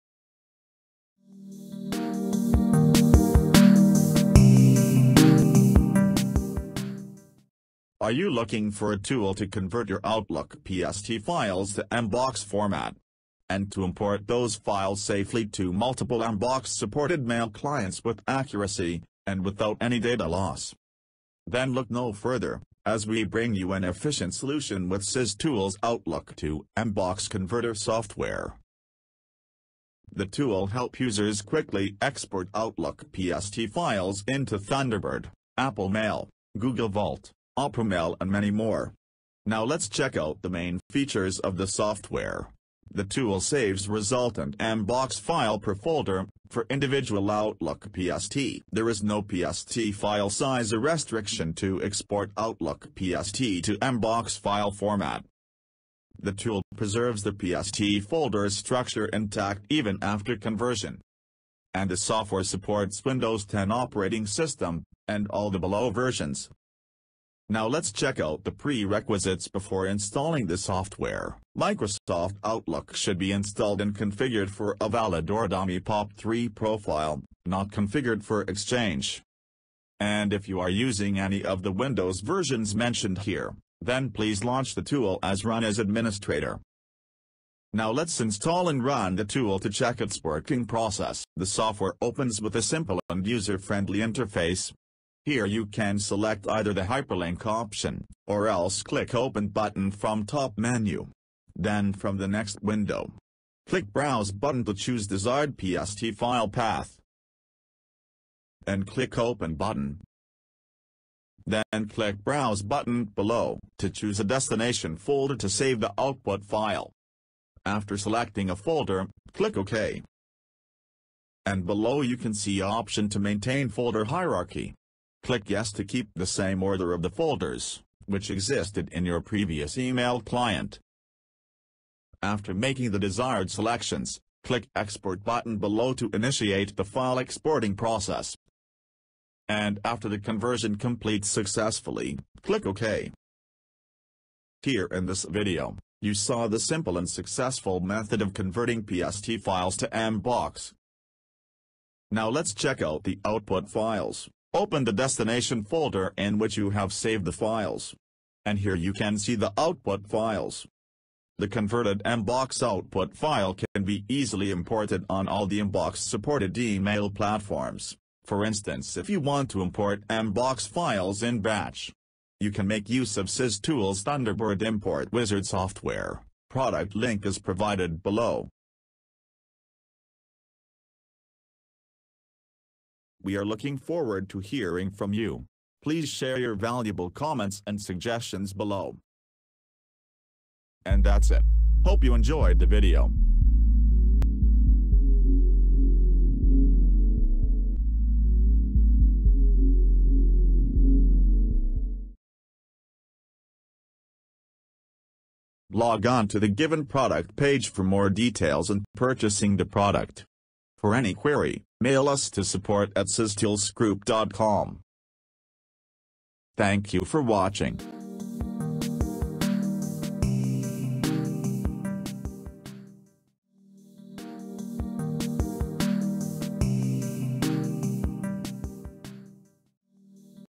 Outlook to MBOX Converter | Convert Outlook PST to MBOX Instantly
With the help of SysTools Outlook PST to MBOX Converter tool, one can smoothly implement the PST file conversion process to the desired MBOX format. Moreover, this risk-free converter tool is capable enough to convert the Outlook emails along with the attachments to MBOX format in a hassle-free way. Besides this, the complete email folders are exported to PST, which includes Inbox, Outbox, Sent Items, Drafts, etc. One of the highlighted features of the software is the ability to create an individual MBOX file for each PST file. Also, this efficient software is designed in such a way that that it can smartly export both ANSI & Unicode file type. Even after migrating the PST file to MBOX format, all the meta-data properties and data integrity of the resultant MBOX file is well-maintained without posing the risk of data loss issue.

One can download the free demo version of the software to undergo a quick demonstration of PST to MBOX conversion process. Moreover, the output MBOX file can be successfully imported to numerous MBOX supported email clients. This includes Apple Mail, Thunderbird, Eudora, Entourage, Netscape, SeaMonkey, Opera Mail and many more. The software is developed with the most advanced algorithms in which it can efficiently import Outlook email with the corresponding attachments. The different attachment file types include a business card(VCF), signature, calendar, inline images and so on. Along with that, the tool renders a simplified and easy to use interface that makes it easy for novice or non-technical users to easily convert the PST file to MBOX format. If in case of any technical help, users can instantly reach out to the technical support team for Instant Service.

Time Stamps :
Features :  0:41
Pre-Requisites : 1:10
Output : 2:32

How it works:
Step 1:  Install & Run SysTools Outlook PST to Mbox Converter Software
Step 2:  Browse & Add Desired PST File
Step 3:  Click Export Button to begin the Export Process
Step 4:  Successfully Export the files